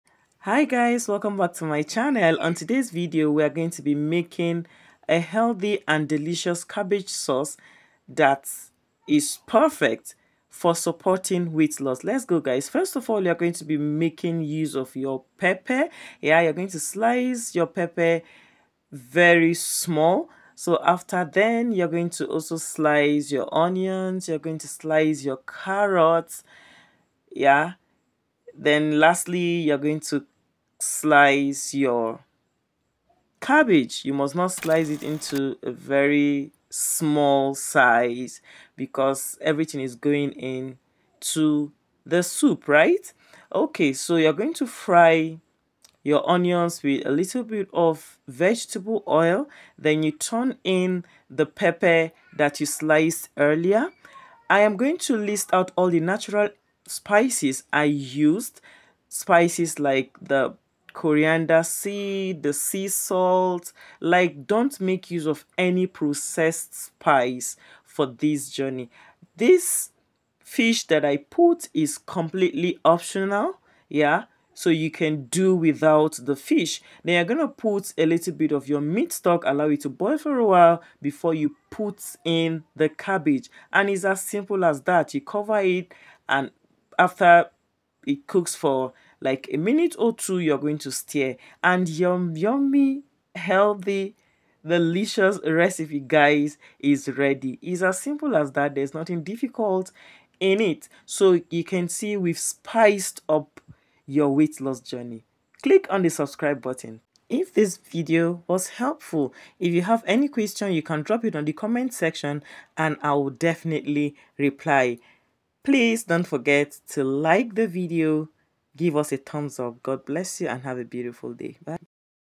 Lose weight fast with cabbage soup formula
Cabbage diet recipe

This recipe is perfect for weight loss in a healthy way

Very simple and easy way to loose weight with just few ingredients

In this YouTube video we outline how to cook even if you are a newbie